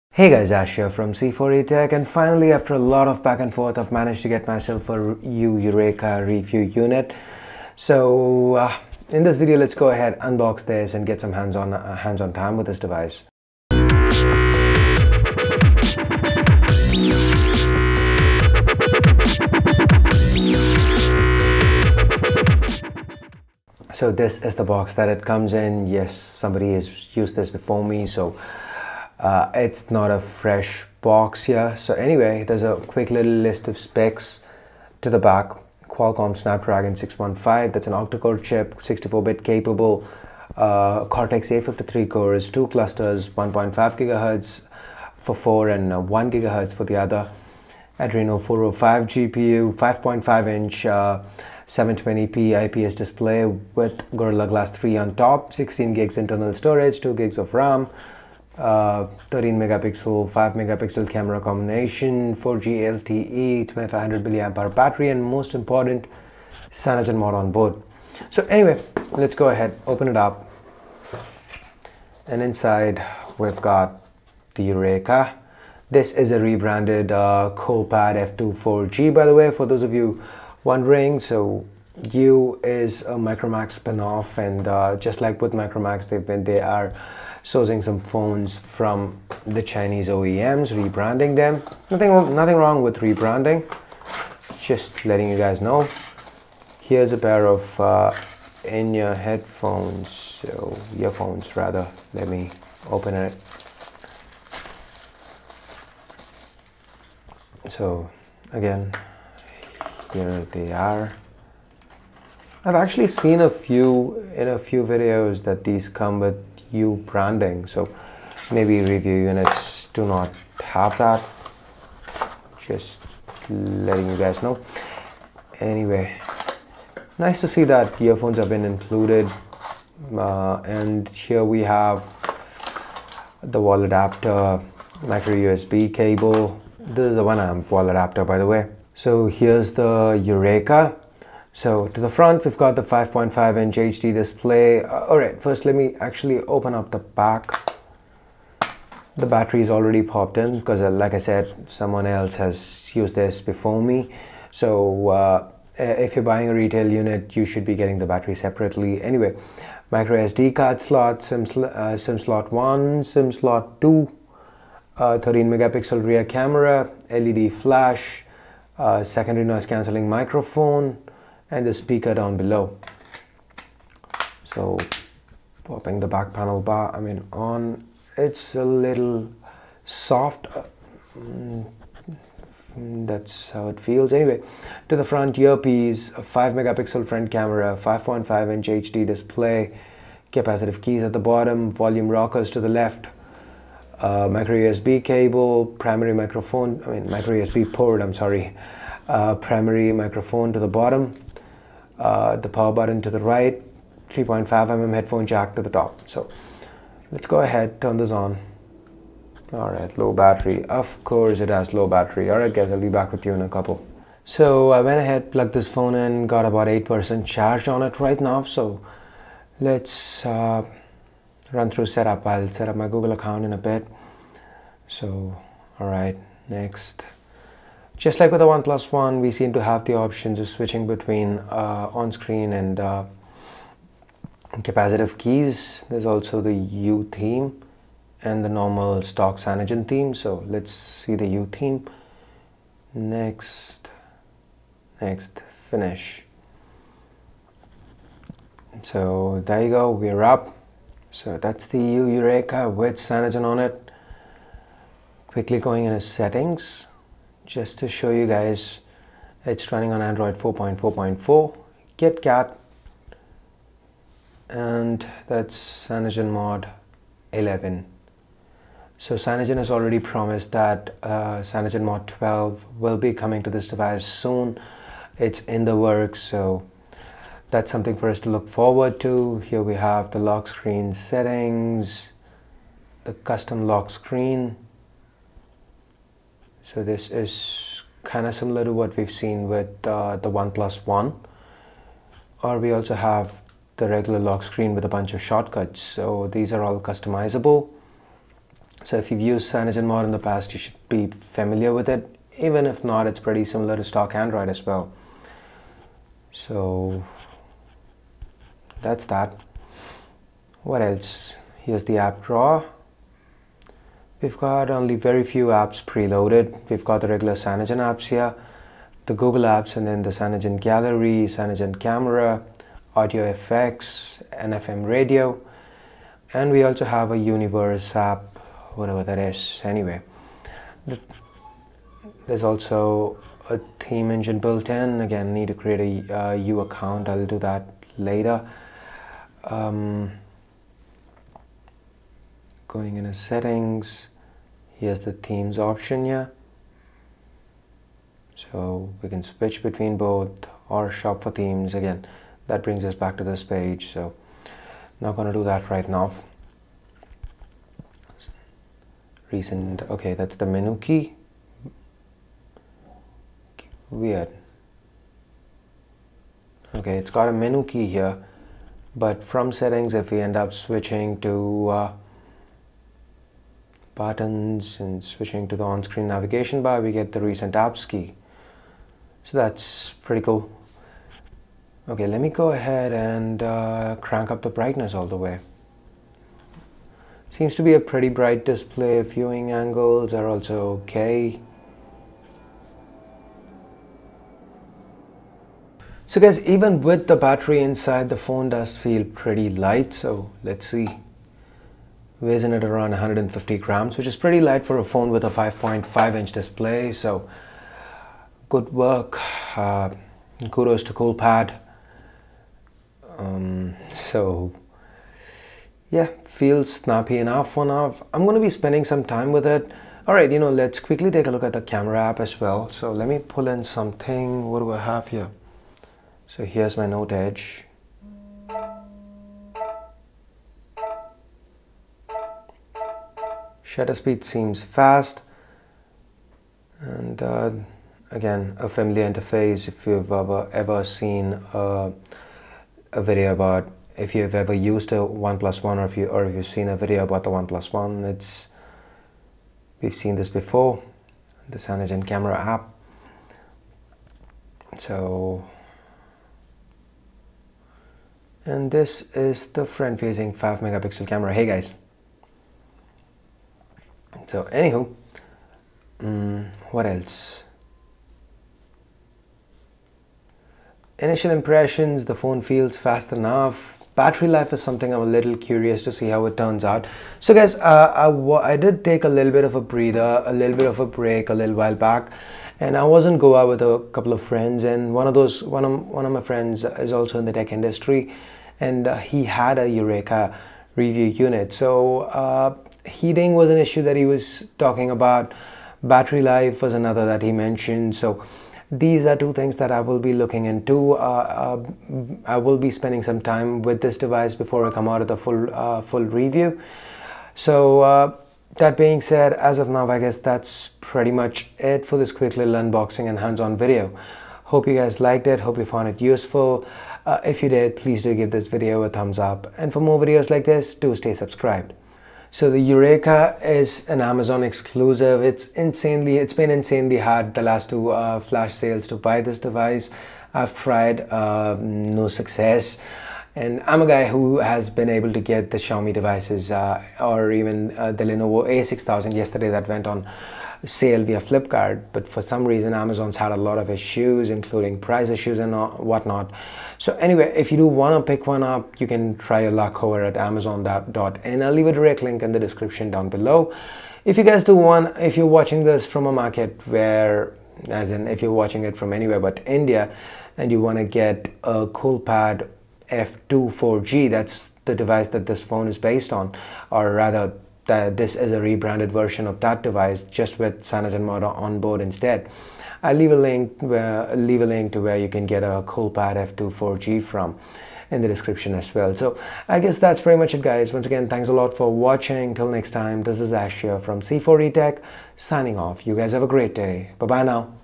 Yu Yureka - Unboxing & Hands On
In this video let's unbox the Yureka from Yu and spend some hands on time with it.

The Yu Yureka is powered by the 64bit capable Snapdragon 615 chipset that houses 8 Cortex A53 cores, 4 clocked at 1.5 Ghz each and another 4 clocked at 1 Ghz each coupled with an Adreno 405 GPU and 2GB RAM.

It has 16GB of internal storage and also a micro sd slot for memory expansion.

The Yureka houses a 13MP Rear Camera capable of capturing 1080p video and has a 5.5" 720p IPS display with a 2500 mAh user replaceable battery...

The Yureka currently runs on Android 4.4 Kitkat with Cyanogenmod 11 on top